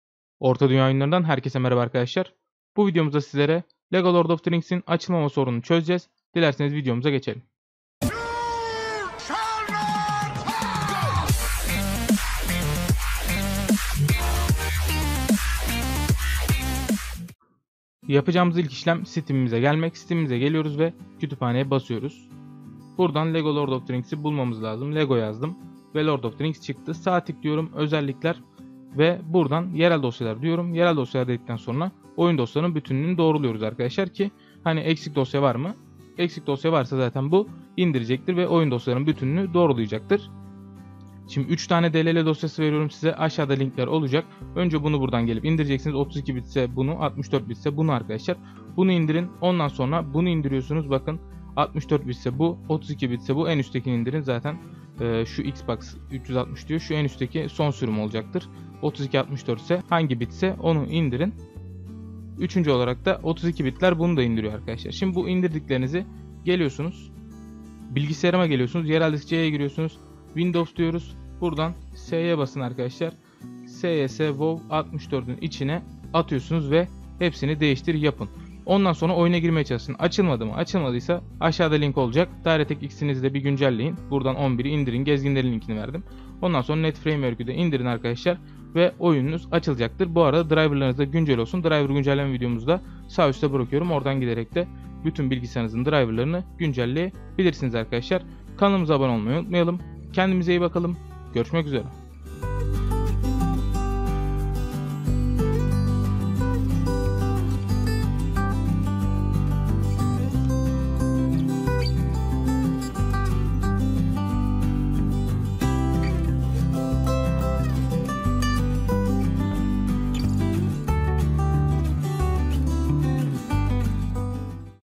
Lego Lord of the Rings Açılmama Sorunu Çözümü
Orta Dünya Oyunlarından Selamlar
Bu videomuzda lego lord of the rings oyununun açılmama sorununu çözdük umarız işe yaramıştır herkese iyi oyunlar.